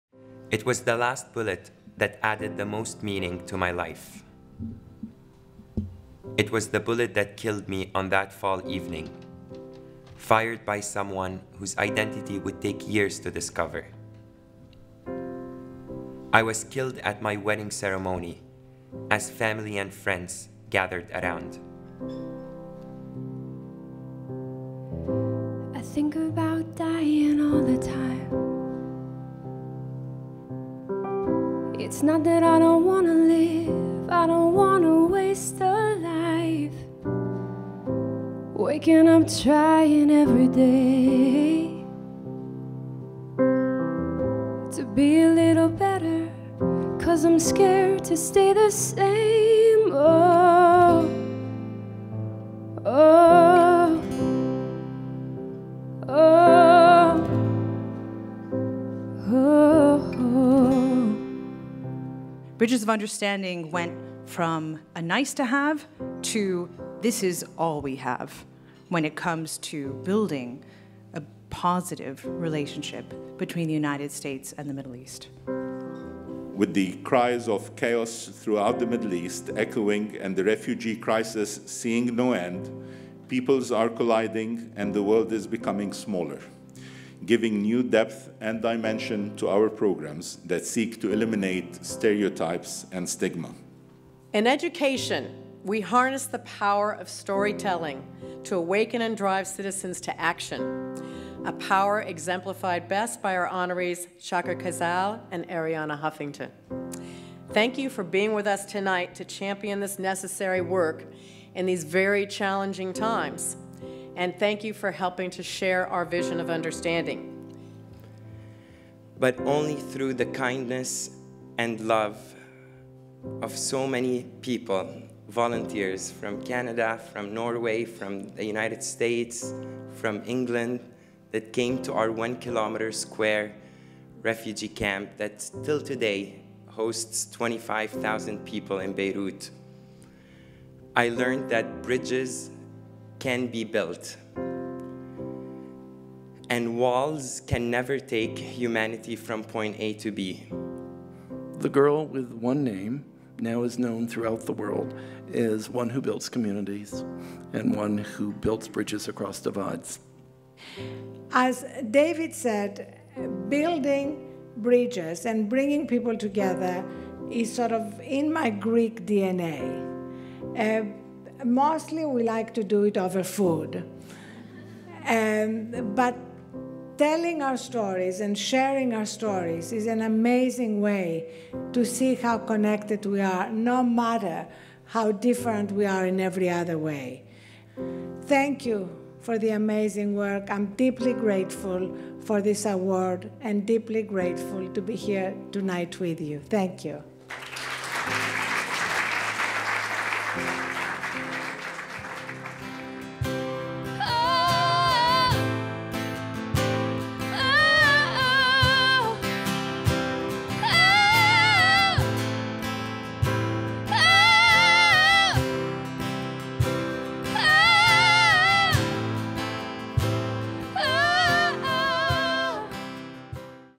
Arianna Huffington and Chaker Khazaal | 2016 Building Bridges Award Tribute
Bridges of Understanding, David G. Bradley (Atlantic Media), and Lara Setrakian (News Deeply) honor Arianna Huffington and Chaker Khazaal at the 2016 Building Bridges Award Dinner. Featuring a performance by recording artist Carrie Manolakos.